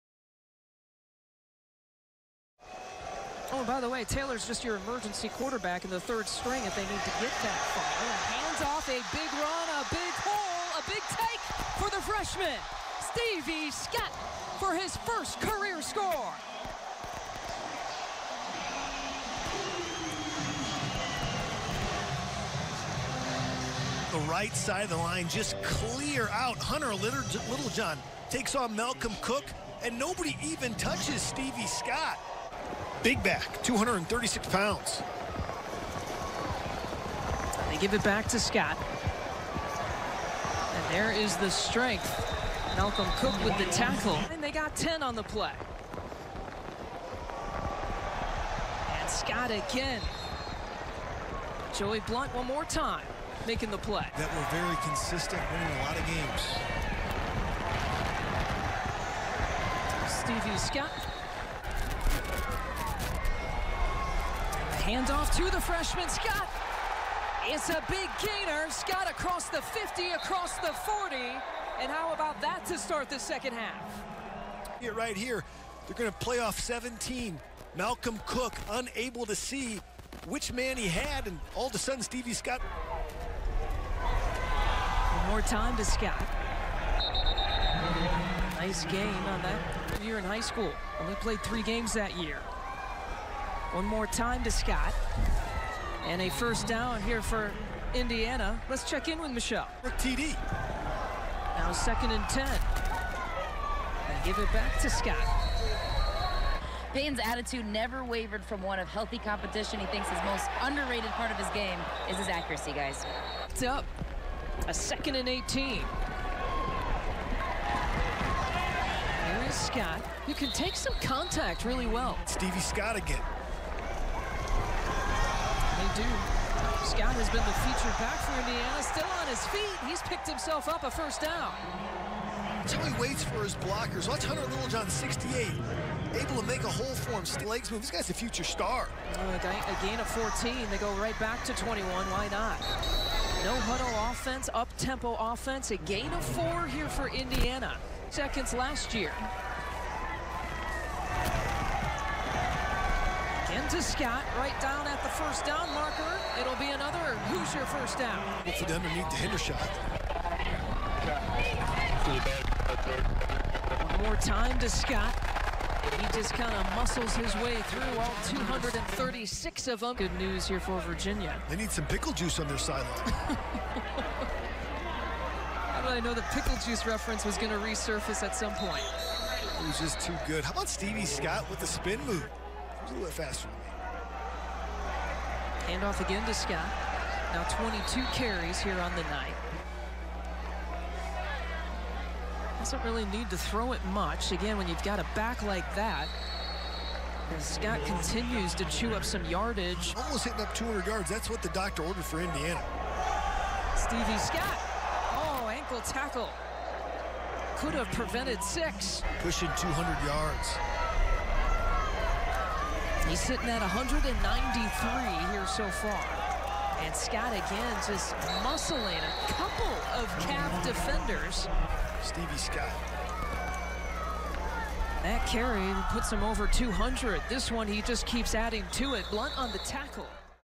#30: Indiana RB Stevie Scott vs. Virginia | Top 30 B1G Football Performances of 2018
Counting down the top performances from Big Ten football in 2018.

At #30 - Indiana RB Stevie Scott

Vs. Virginia: 31 carries, 204 yards, one touchdown.